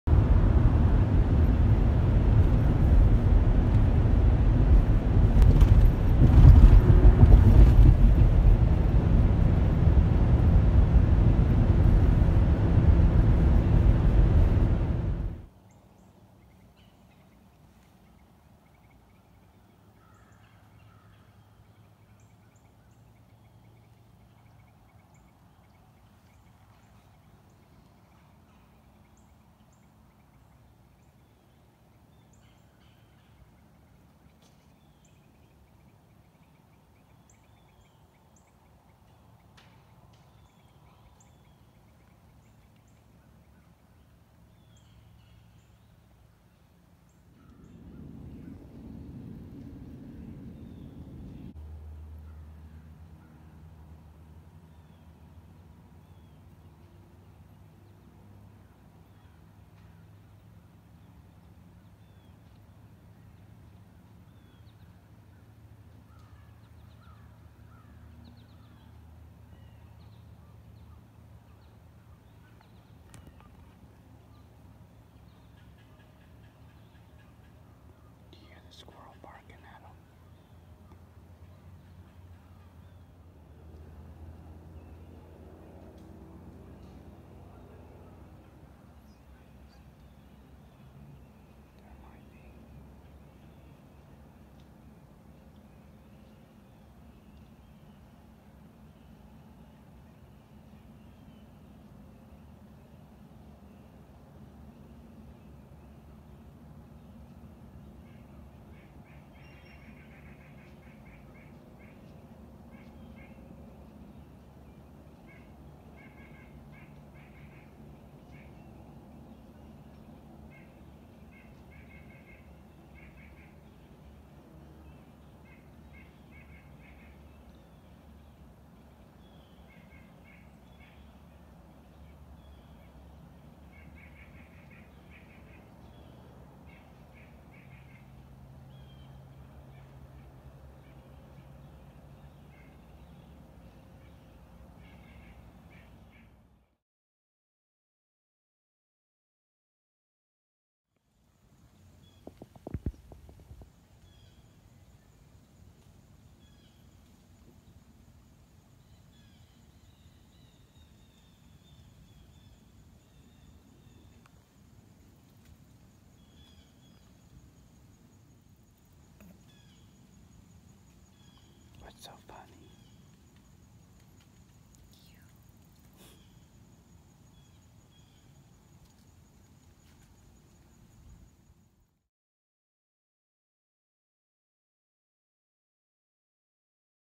My wife’s first time behind the bow!
I took my wife down to the property I deer hunt in Southern Indiana. She didn’t connect but had opportunities that she chose not to take. Great day to be out in the woods!

3 bucks and 2 does